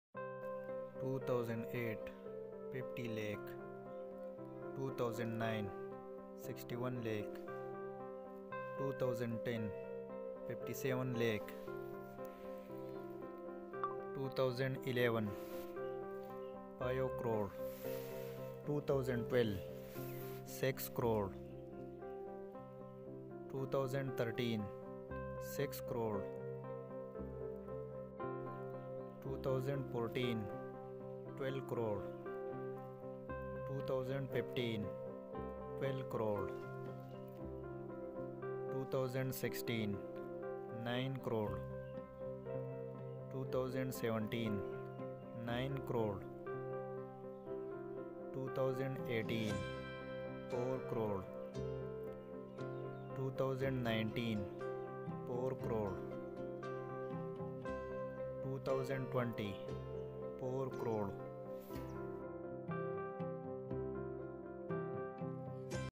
Shane Watson IPL Salary Per Seasons 2008-2020 | Shane Watson IPL Income
In this video we shared Shane Watson IPL Salary Per Seasons from 2008 to 2020
He Played for 3 Teams in IPL History Rajasthan Royals, Royal Challengers Banglore and Chennai Super Kings